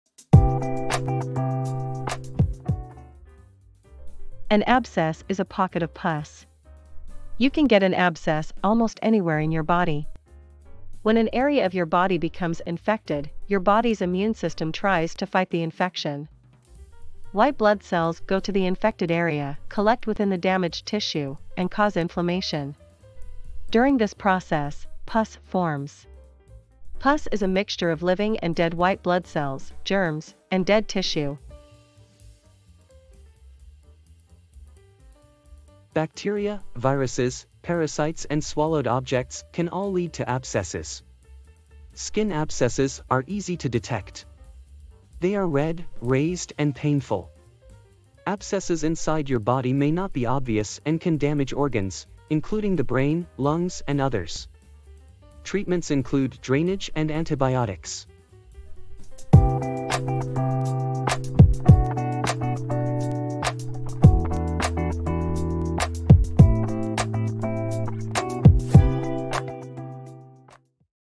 Abscess - Causes, Symptoms and Treatment
An abscess is a pocket of pus. You can get an abscess almost anywhere in your body. When an area of your body becomes infected, your body's immune system tries to fight the infection. White blood cells go to the infected area, collect within the damaged tissue, and cause inflammation. During this process, pus forms. Pus is a mixture of living and dead white blood cells, germs, and dead tissue.

Bacteria, viruses, parasites and swallowed objects can all lead to abscesses. Skin abscesses are easy to detect. They are red, raised and painful. Abscesses inside your body may not be obvious and can damage organs, including the brain, lungs and others. Treatments include drainage and antibiotics.